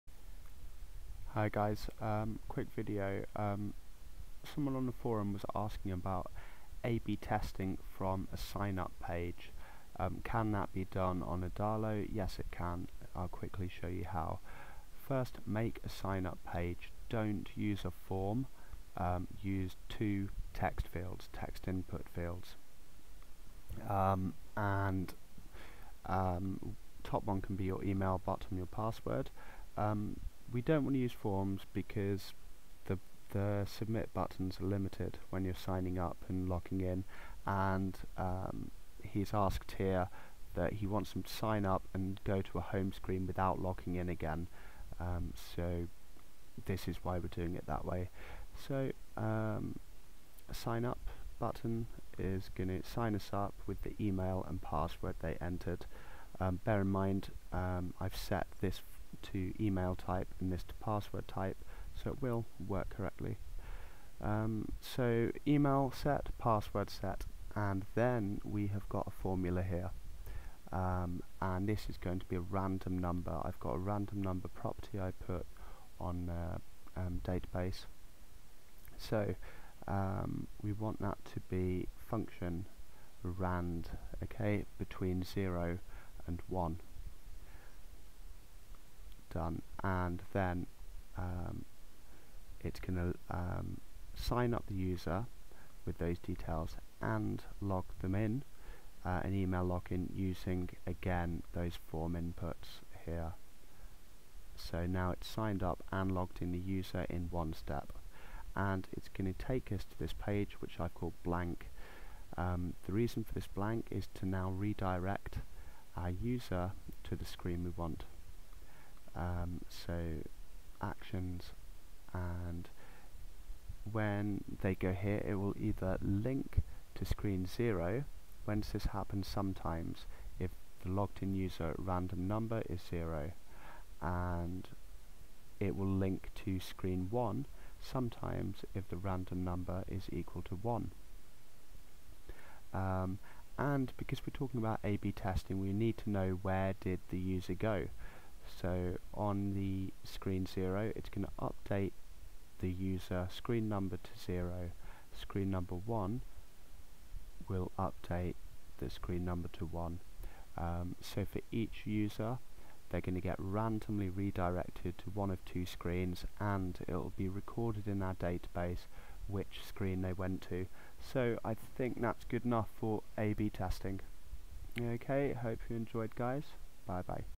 Adalo A/B Testing from sign up screen
How to set up A/B testing in an Adalo app.

More features for your Adalo app
- Better file uploader
- Detect User Device
- Twilio SMS
- String Functions